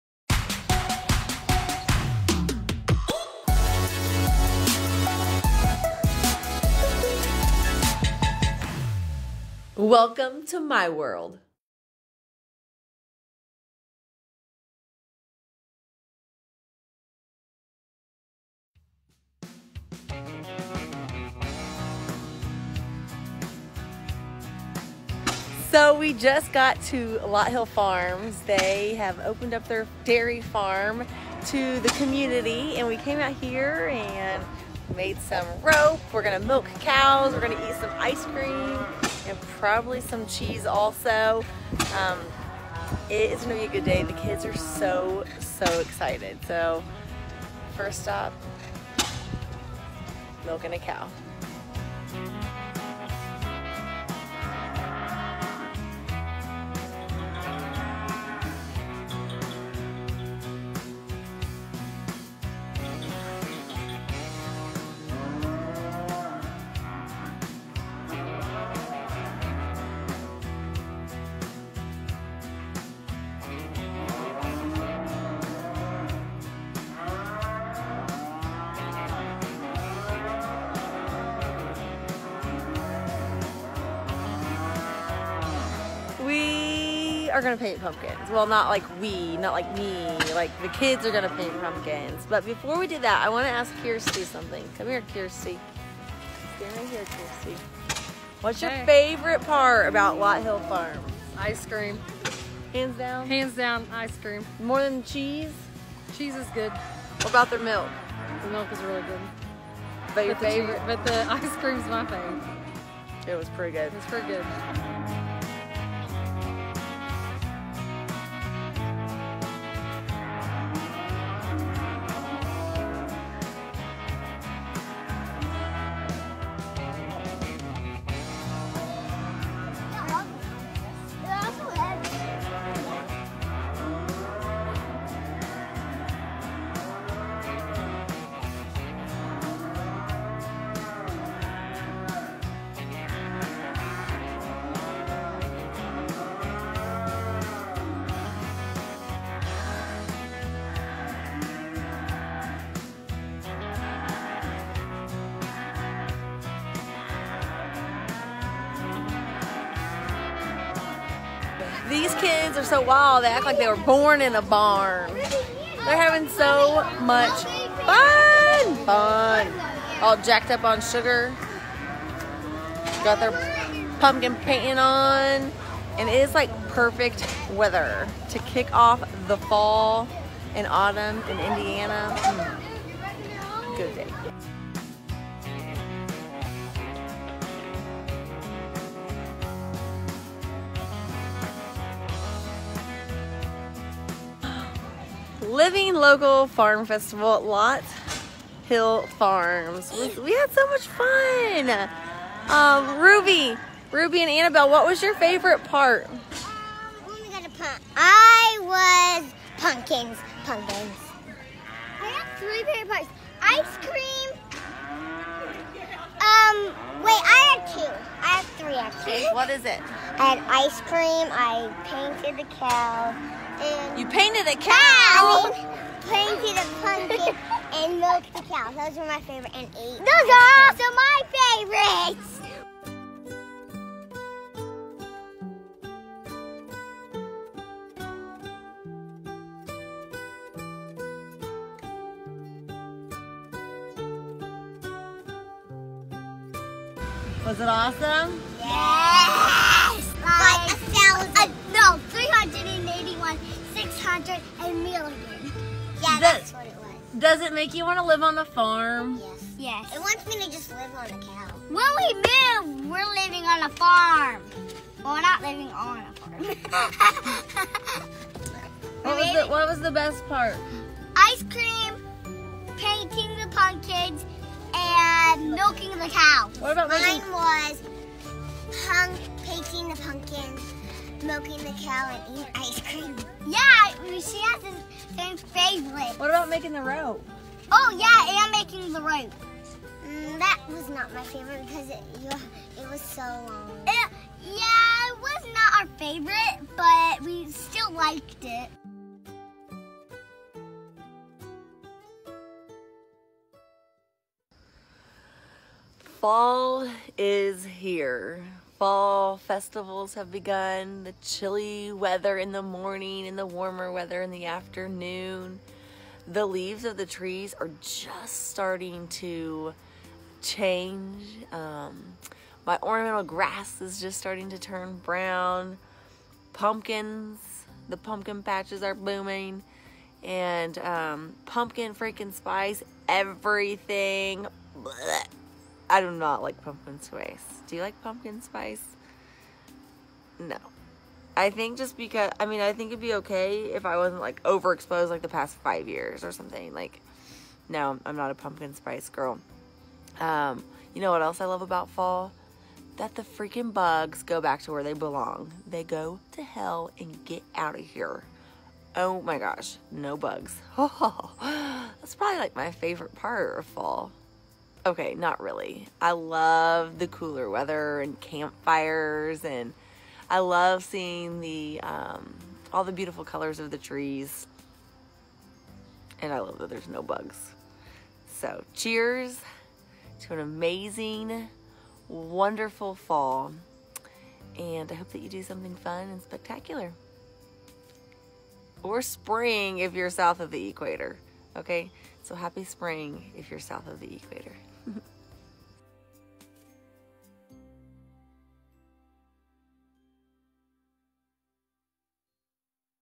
Indiana Dairy Farm
I love a lot of things about fall, the changing leaves, the crisp weather and that the bugs go back to where they belong. HELL, that is exactly where bugs belong!
The living local farm festival at Lot Hill Dairy Farm was a blast. From the tractor show, car show, petting th eanimals, miliing a cow, all the ice cream you can imagine and painting pumpkins. We kicked off this fall with a bang!